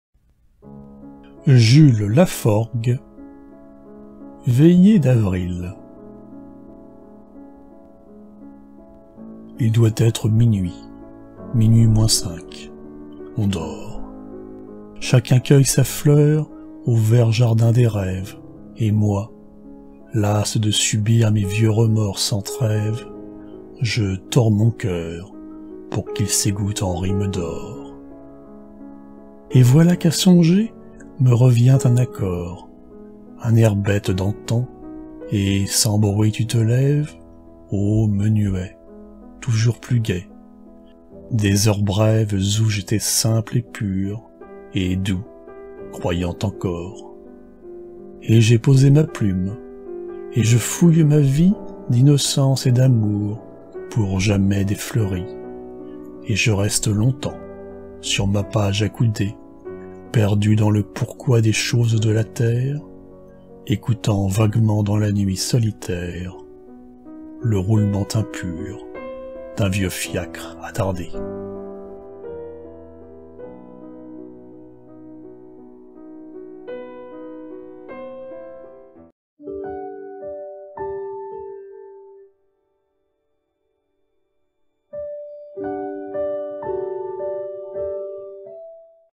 Veillée d'avril - Sonnet - Jules Laforgue
Veillée d'avril
Poésie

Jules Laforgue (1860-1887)

Lecture : AB
Portrait de couverture : Franz Skarbina
Illustration de fond : Pierre Puvis de Chavannes

Texte :
Il doit être minuit. Minuit moins cinq. On dort.
Chacun cueille sa fleur au vert jardin des rêves,
Et moi, las de subir mes vieux remords sans trêves
Je tords mon cœur pour qu'il s'égoutte en rimes d'or.

Et voilà qu'à songer, me revient un accord,
Un air bête d'antan, et sans bruit tu te lèves
Ô menuet, toujours plus gai, des heures brèves
Où j'étais simple et pur, et doux, croyant encor.

Et j'ai posé ma plume. Et je fouille ma vie
D'innocence et d'amour pour jamais défleurie,
Et je reste longtemps, sur ma page accoudé,

Perdu dans le pourquoi des choses de la terre,
Écoutant vaguement dans la nuit solitaire
Le roulement impur d'un vieux fiacre attardé.